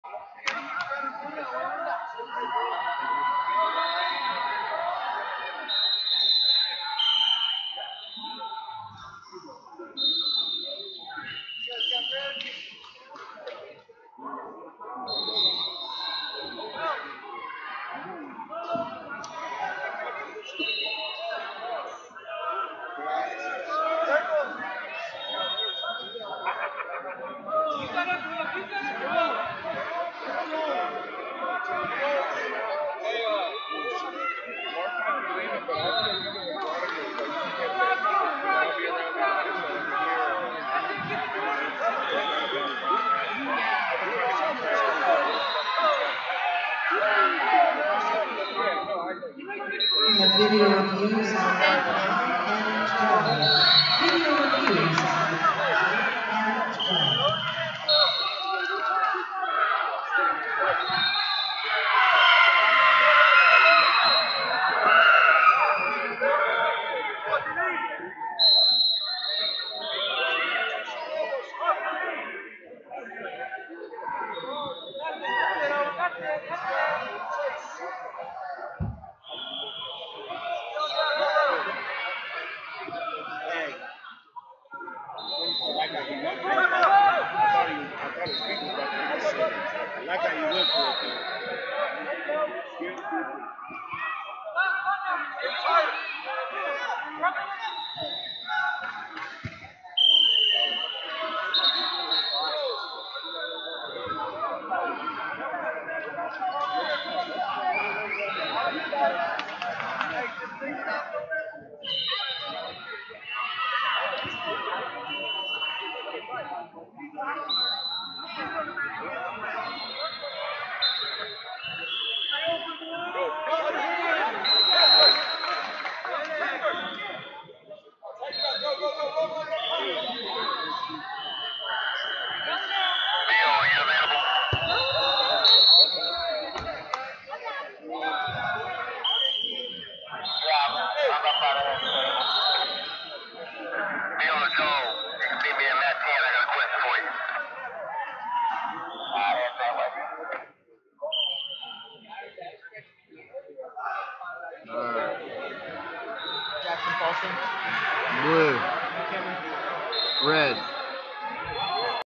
114 Lbs Round 3 - Brooks McCollom, OK Vs Owen Burns, OK 6876
Flosports-partner-access/2023 Southern Plains Regional Championships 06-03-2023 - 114 Lbs Round 3 - Brooks McCollom, OK Vs Owen Burns, OK 6876